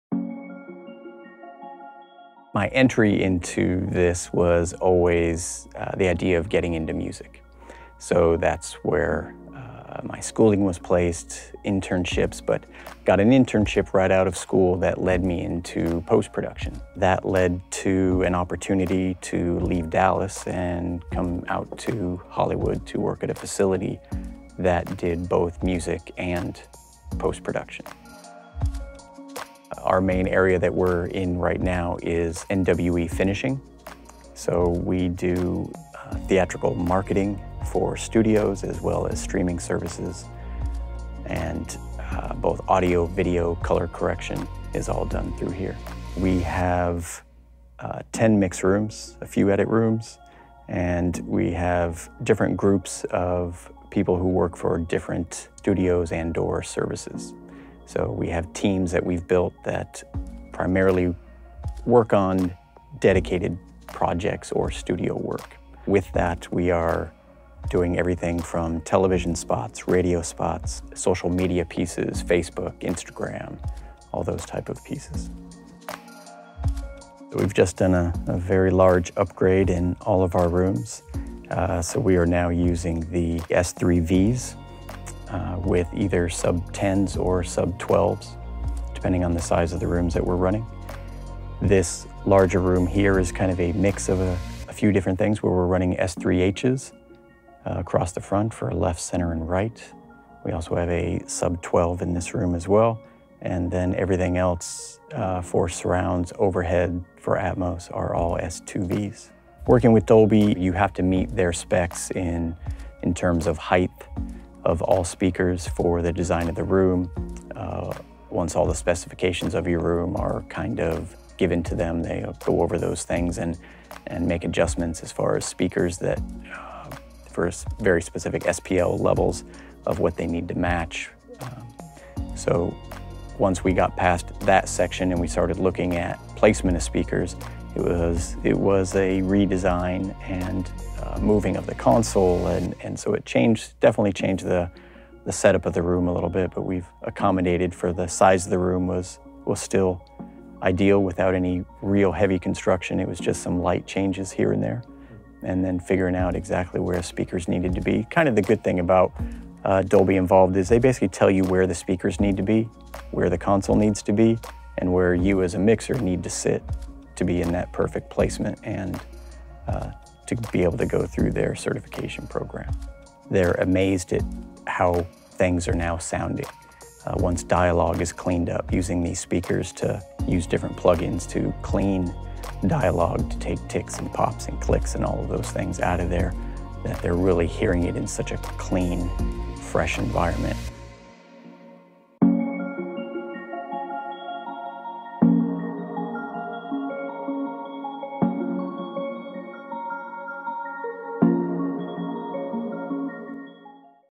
ADAM Audio - IN THE STUDIO with New Wave Entertainment (Dolby Atmos)
ADAM Audio sits down with Mark Rodrigues, Chief Engineer at New Wave Entertainment, to discuss why they chose the ADAM Audio S Series for their Dolby Atmos room in their Los Angeles studio. New Wave Entertainment utilizes ADAM Audio S3H as the LCR and ADAM Audio S2V for the surrounds.